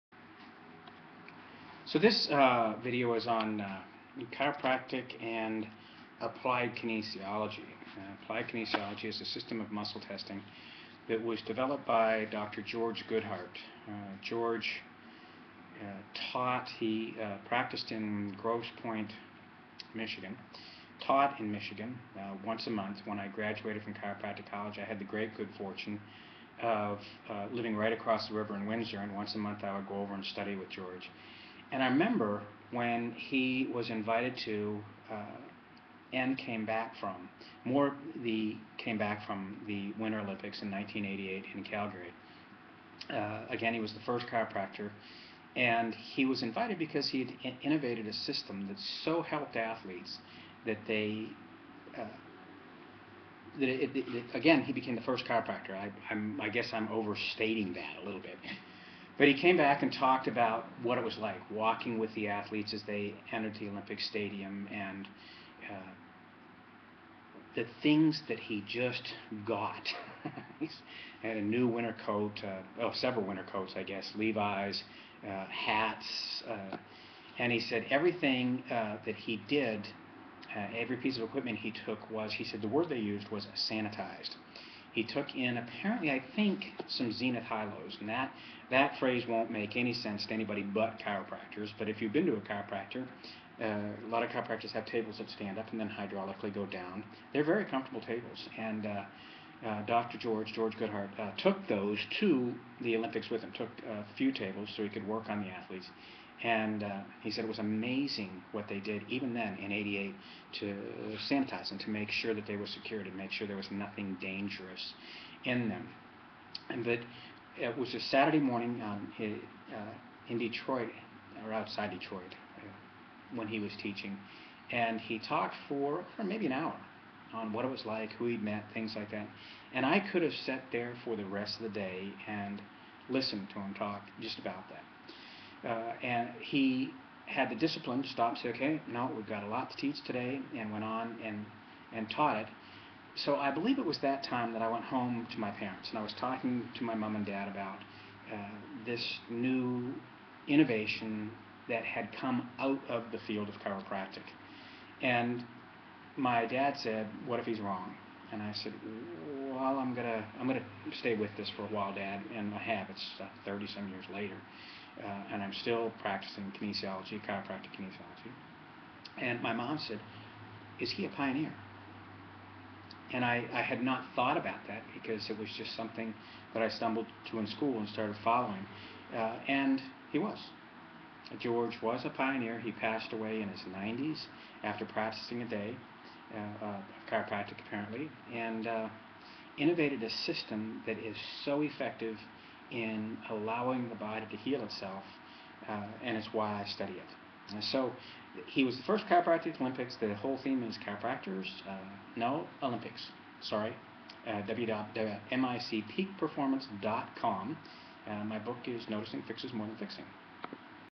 The Olympics & Applied Kinesiology • George Goodheart & Mick MacKenzie
George Goodheart - Pioneer of Natural Medicine - Father of Applied Kinesiology - was the first Chiropractor to be invited to the Winter Olympics - Calgary, 1988...